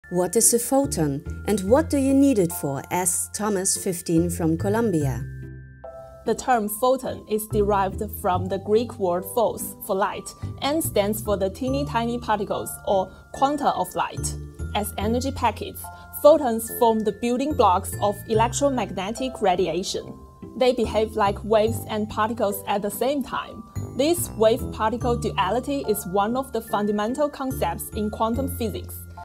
What is a photon? // Was ist ein Photon? - 🎥 QUANTube #7
Why is nothing faster than light? And how long are sunrays? In the seventh episode of the QUANTube video series, Shiyu answers all questions about superfast light and photonics, i.e. the use of optical technologies and light signals. In the process, the doctoral student reveals what stealth has to do with physics and why we are living in the century of photonics. “QUANTube – Science Break” is a video series connected to the mobile game created by the Cluster of Excellence ct.qmat.

Warum ist nichts schneller als das Licht? Und wie lang sind eigentlich Sonnenstrahlen? In der siebten Folge der QUANTube-Videoreihe beantwortet Shiyu alle Fragen rund um das superschnelle Licht und die Photonik, also die Nutzung von optischen Technologien und Lichtsignalen. Dabei verrät die Doktorandin, was Tarnkappen mit Physik zu tun haben und warum wir im Jahrhundert der Photonik leben. „QUANTube – kurze Pause Wissenschaft“ ist die Videoreihe zum Handyspiel des Exzellenzclusters ct.qmat und widmet sich den Fragen der Spieler:innen zu quantenphysikalischen Phänomenen.